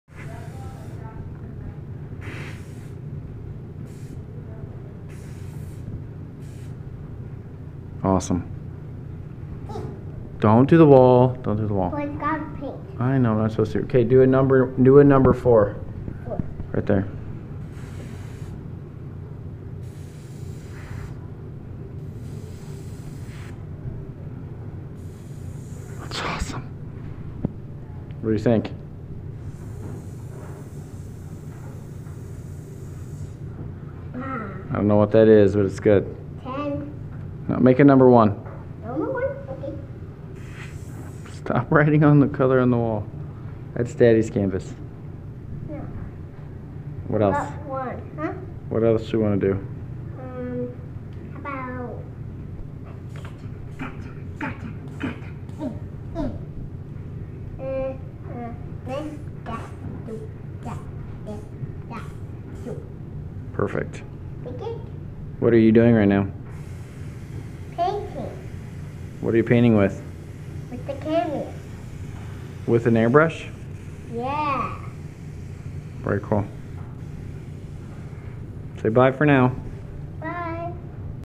4 year old Airbrushing
Otto is a four year old air brushing for the first time.